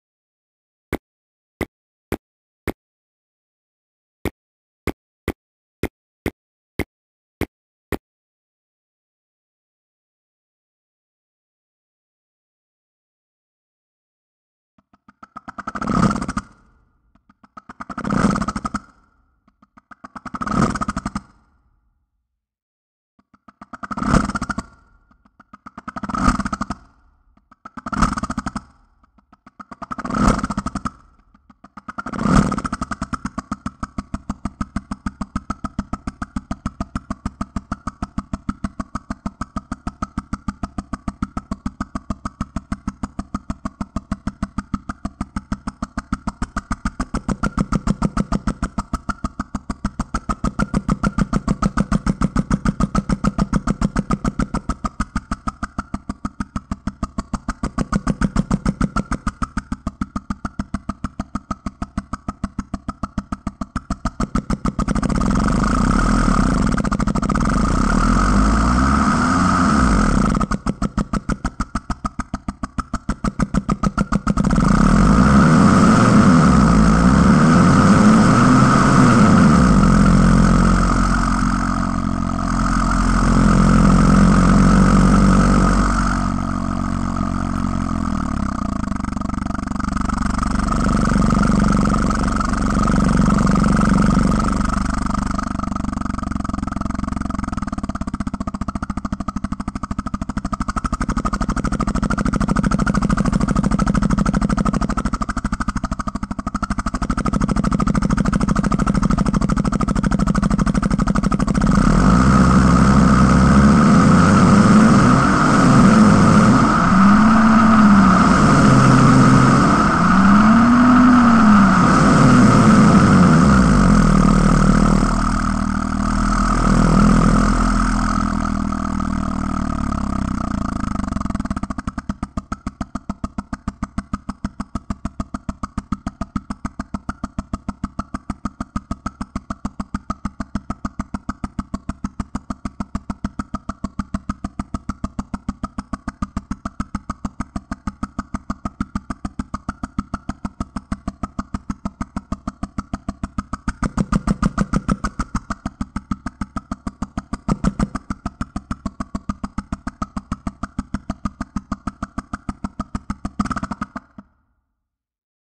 The Mouth Engine Mobile — Metasound
The Mouth Engine Mobile is a silly real-time car-driving experiment in which all the car sounds are made with my mouth. It was made in Unreal.

This video focuses on how I used Metasound to make a goofy — though somewhat realistic — engine sound with simple voice recordings of just one impulse each. There is no pitch shifting of the samples based on the BPM; the pitch comes naturally as the samples are triggered at high frequencies.